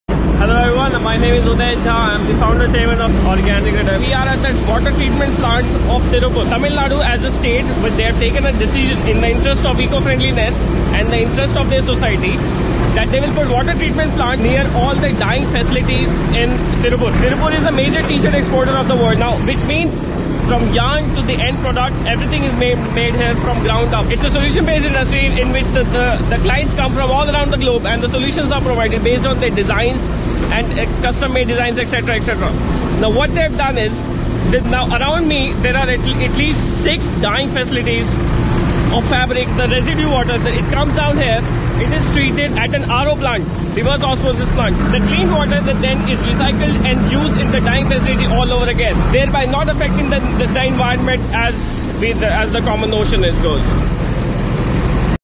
Water recycling plant in TIRUPPUR
here is your look at the water treatment plant of the tshirts hub of south india, Tiruppur in Tamil Nadu. amazing place to be, very well maintained.